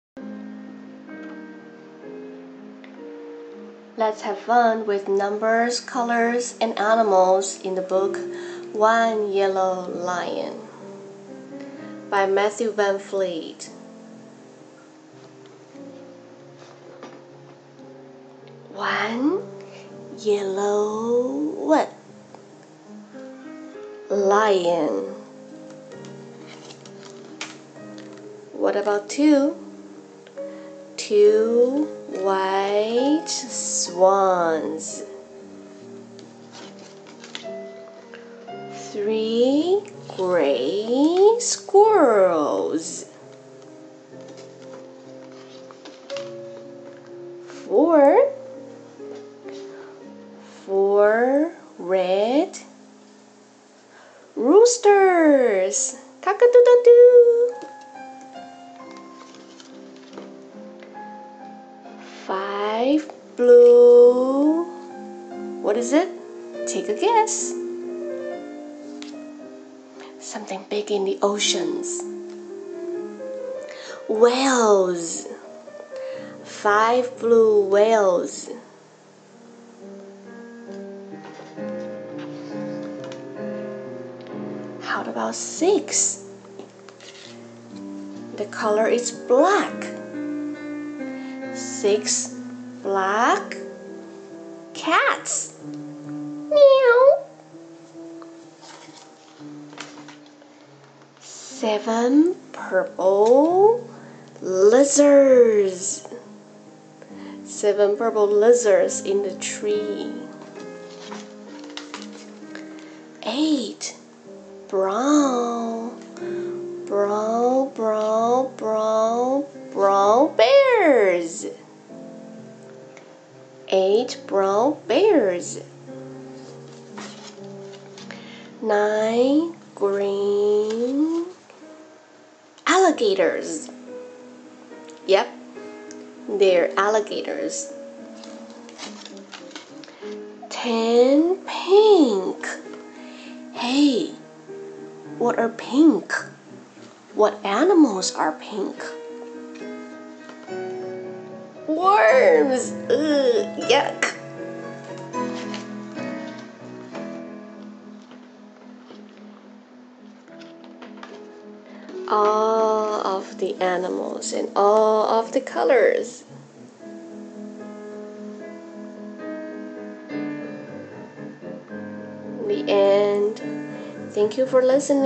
One Yellow Lion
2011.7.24 Teacher Catherine read the picture book.